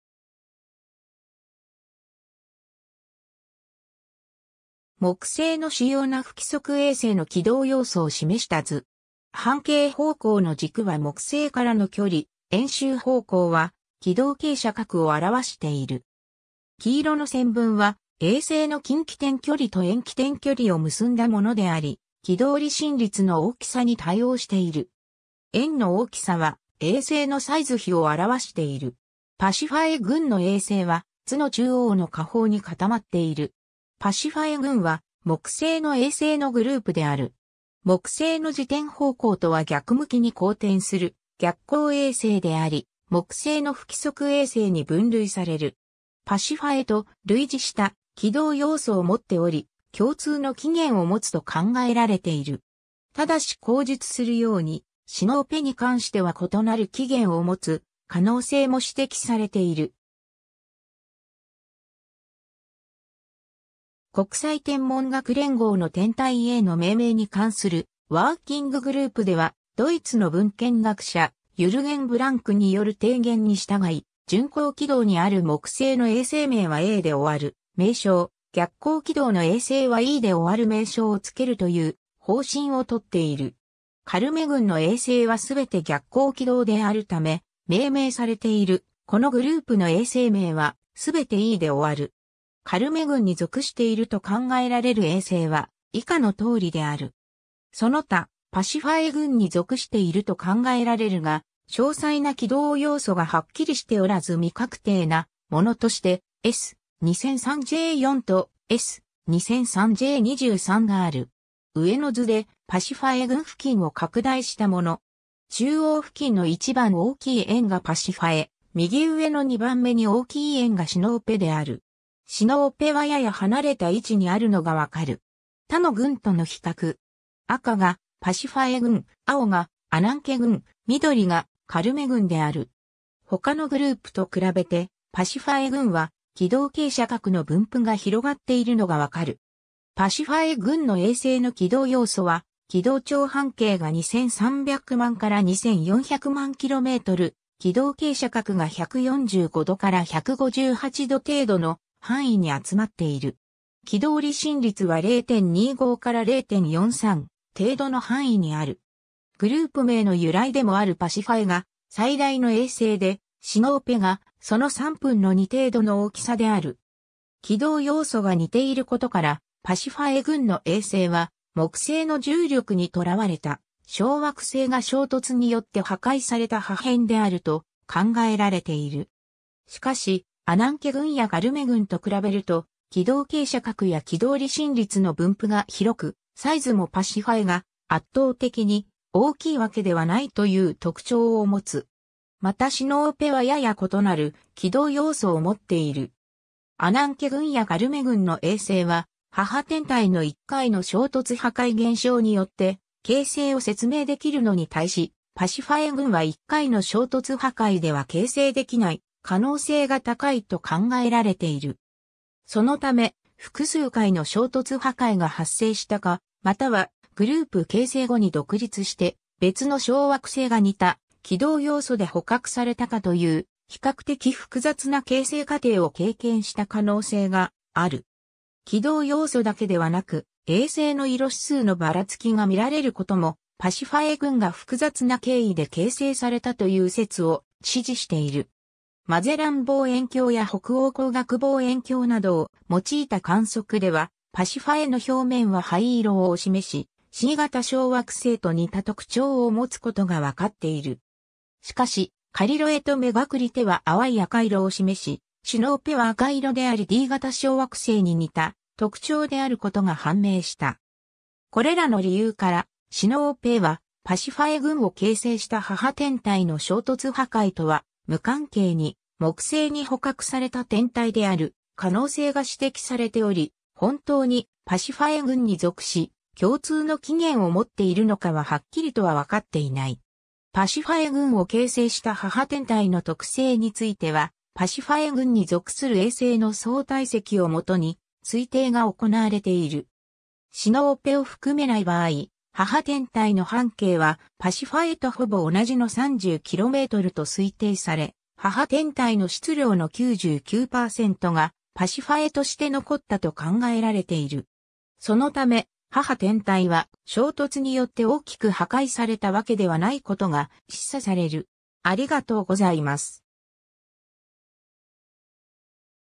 パシファエ群
木星の主要な不規則衛星の軌道要素を示した図。
半径方向の軸は木星からの距離、円周方向は軌道傾斜角を表している。
黄色の線分は衛星の近木点距離と遠木点距離を結んだものであり、軌道離心率の大きさに対応している。
円の大きさは衛星のサイズ比を表している。
パシファエ群の衛星は図の中央の下方に固まっている。
 パシファエ群（パシファエぐん、英語：Pasiphae group）は、木星の衛星のグループである。
木星の自転方向とは逆向きに公転する逆行衛星であり、木星の不規則衛星に分類される。
パシファエと類似した軌道要素を持っており、共通の起源を持つと考えられている。
ただし後述するように、シノーペに関しては異なる起源を持つ可能性も指摘されている。
 国際天文学連合の天体への命名に関するワーキンググループでは、ドイツの文献学者 Jürgen Blunck による提言に従い、順行軌道にある木星の衛星名は a で終わる名称、逆行軌道の衛星は e で終わる名称を付けるという方針を取っている。
カルメ群の衛星は全て逆行軌道であるため、命名されているこのグループの衛星名は全て e で終わる。
 カルメ群に属していると考えられる衛星は以下の通りである。
 その他、パシファエ群に属していると考えられるが、詳細な軌道要素がはっきりしておらず未確定なものとして、S/2003 J 4とS/2003 J 23がある。
 上の図でパシファエ群付近を拡大したもの。
中央付近の一番大きい円がパシファエ、右上の2番目に大きい円がシノーペである。
シノーペはやや離れた位置にあるのが分かる。
 他の群との比較。
赤がパシファエ群、青がアナンケ群、緑がカルメ群である。
他のグループと比べてパシファエ群は軌道傾斜角の分布が広がっているのが分かる。
 パシファエ群の衛星の軌道要素は、軌道長半径が2300万〜2400万km、軌道傾斜角が 145°〜158° 程度の範囲に集まっている。
軌道離心率は0.25〜0.43程度の範囲にある。
グループ名の由来でもあるパシファエが最大の衛星で、シノーペがその3分の2程度の大きさである。
 軌道要素が似ていることから、パシファエ群の衛星は木星の重力にとらわれた小惑星が衝突によって破壊された破片であると考えられている。
しかし、アナンケ群やカルメ群と比べると軌道傾斜角や軌道離心率の分布が広く (図参照)、サイズもパシファエが圧倒的に大きいわけではない (シノーペはパシファエの6割強のサイズ) という特徴を持つ。
またシノーペはやや異なる軌道要素を持っている。
 アナンケ群やカルメ群の衛星は母天体の一回の衝突破壊現象によって形成を説明できるのに対し、パシファエ群は一回の衝突破壊では形成できない可能性が高いと考えられている。
そのため、複数回の衝突破壊が発生したか、またはグループ形成後に独立して別の小惑星が似た軌道要素で捕獲されたかという、比較的複雑な形成過程を経験した可能性がある。
 軌道要素だけではなく、衛星の色指数のばらつきが見られることも、パシファエ群が複雑な経緯で形成されたという説を支持している。
マゼラン望遠鏡や北欧光学望遠鏡などを用いた観測では、パシファエの表面は灰色を示し、C型小惑星と似た特徴を持つことが分かっている。
しかしカリロエとメガクリテは淡い赤色を示し、シノーペは赤色でありD型小惑星に似た特徴であることが判明した。
これらの理由から、シノーペはパシファエ群を形成した母天体の衝突破壊とは無関係に木星に捕獲された天体である可能性が指摘されており、本当にパシファエ群に属し、共通の起源を持っているのかははっきりとは分かっていない。
 パシファエ群を形成した母天体の特性については、パシファエ群に属する衛星の総体積をもとに推定が行われている。
シノーペを含めない場合、母天体の半径はパシファエとほぼ同じの 30 km と推定され、母天体の質量の 99% がパシファエとして残ったと考えられている (シノーペを含めた場合は 79%)。
そのため、母天体は衝突によって大きく破壊されたわけではないことが示唆される。